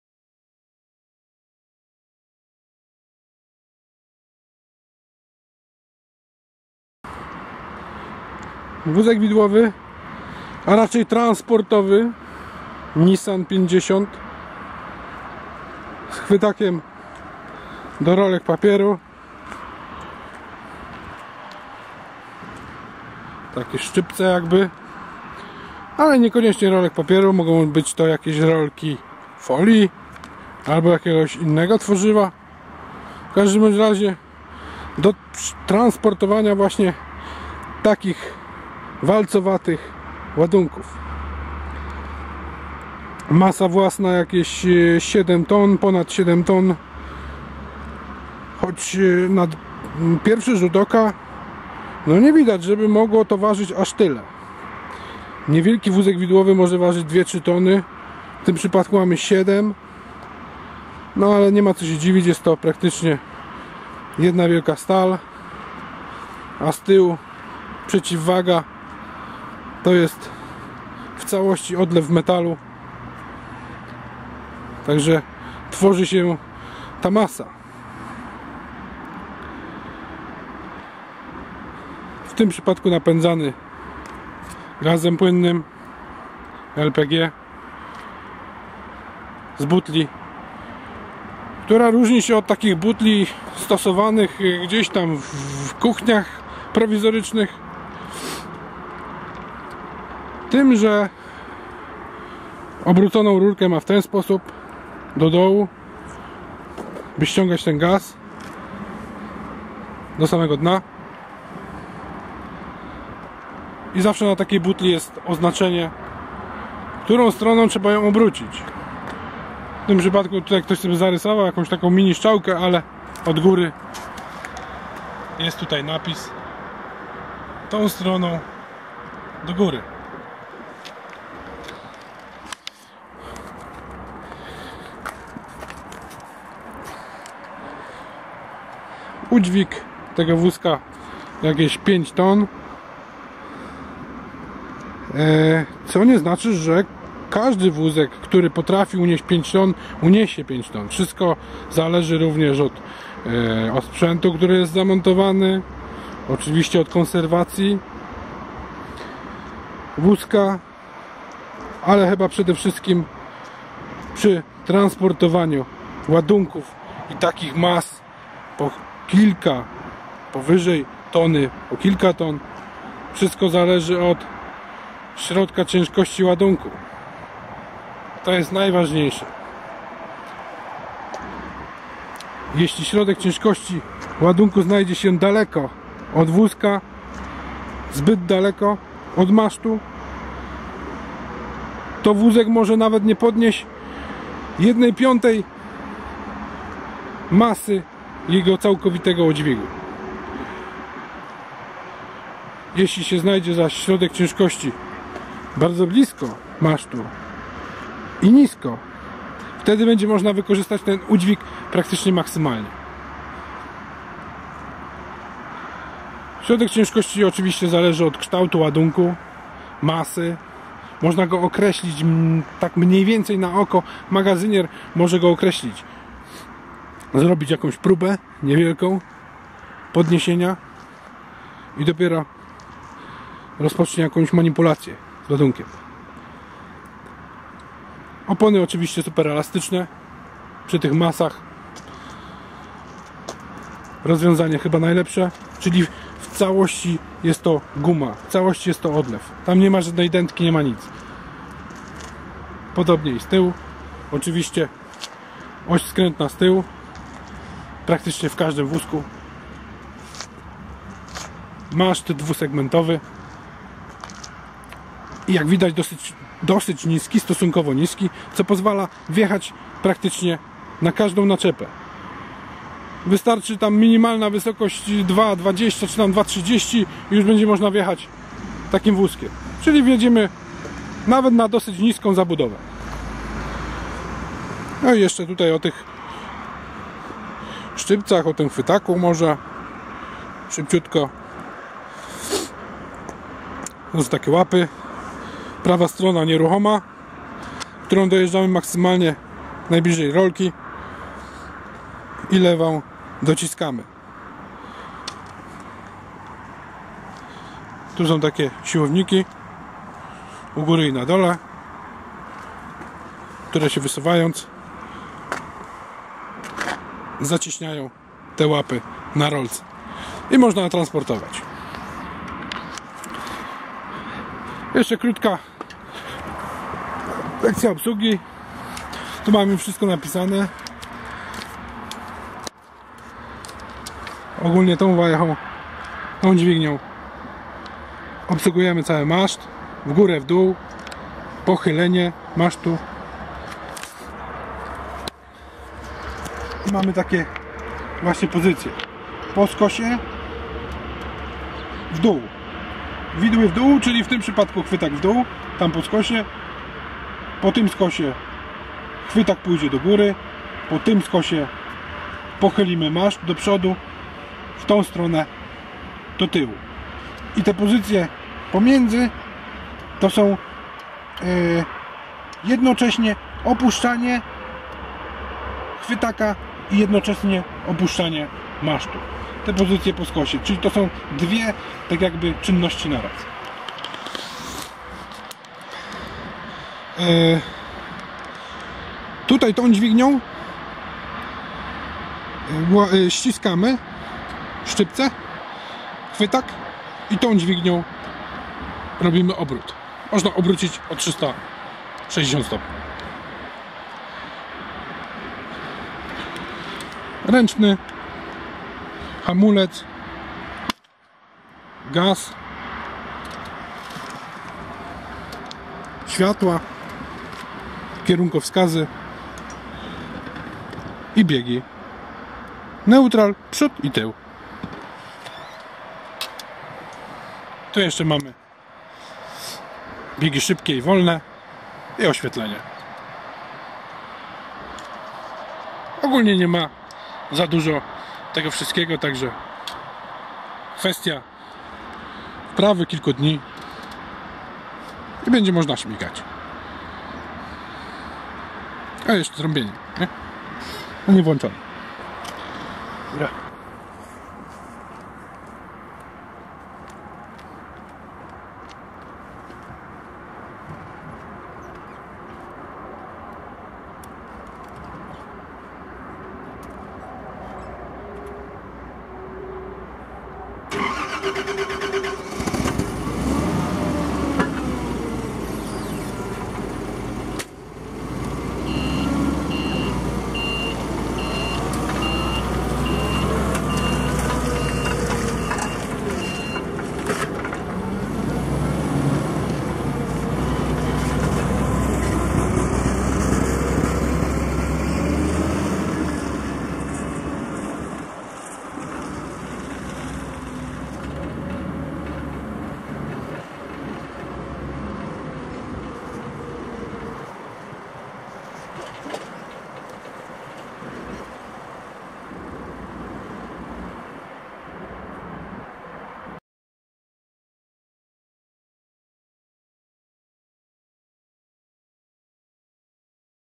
Wózek Nissan 50 z chwytakiem rolek
Prezentacja wózka widłowego z chwytakiem do rolek papieru.